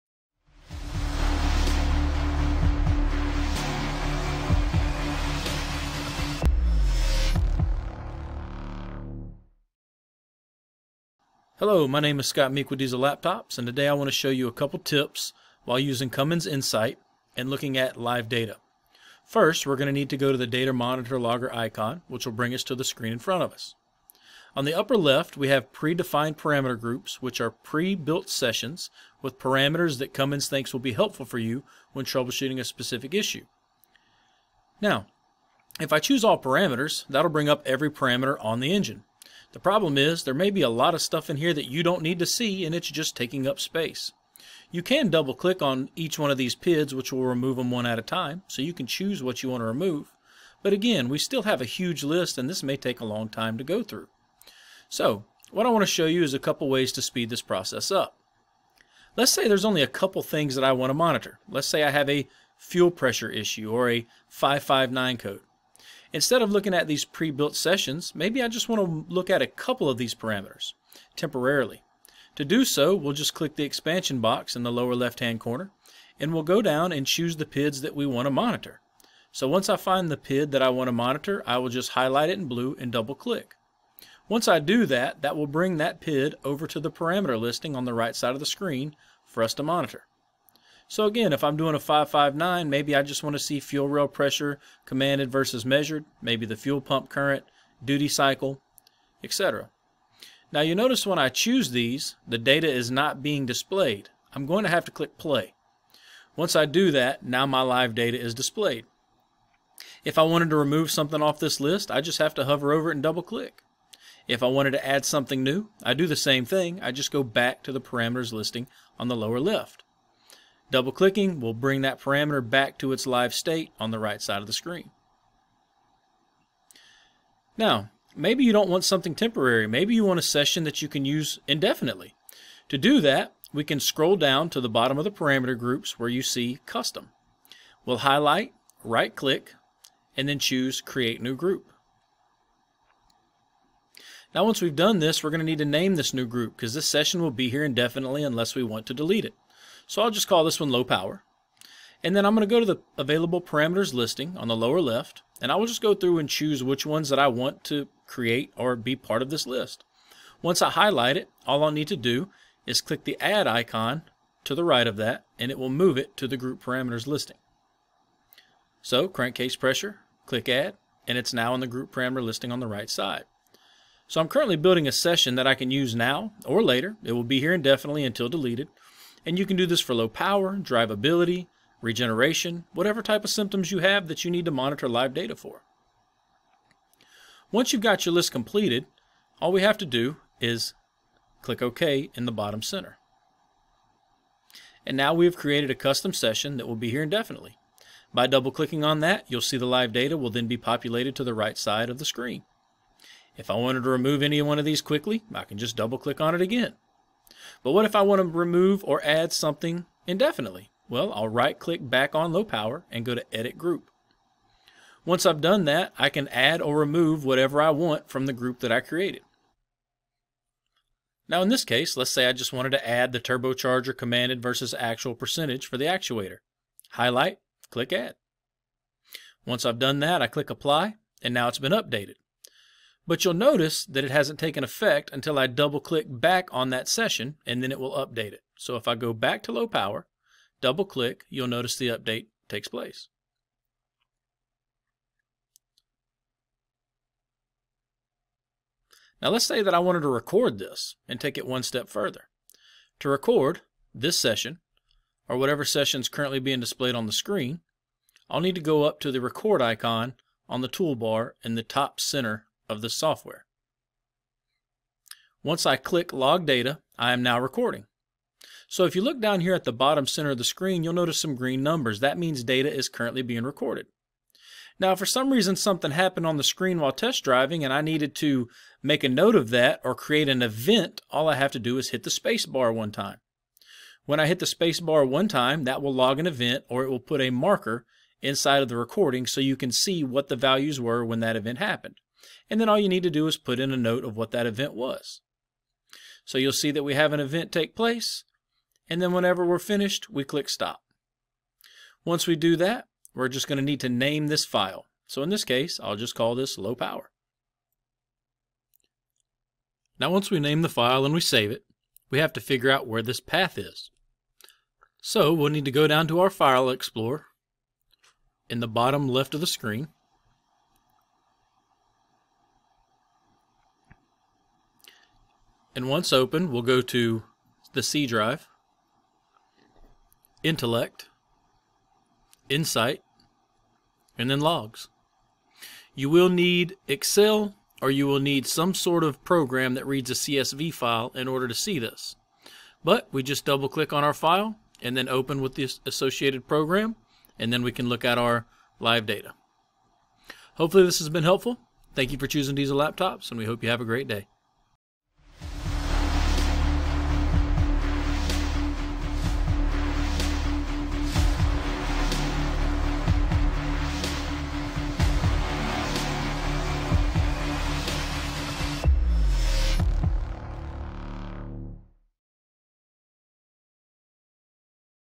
Cummins Insite Time-Saving Tips!
Are you tired of spending a countless amount of time sifting through Cummins Insite for one data point? Let us help you save time and money with this 6-minute Tech Tip!

Stuck on a repair? Get tech docs, troubleshooting guides and so much more with your free Diesel Repair account: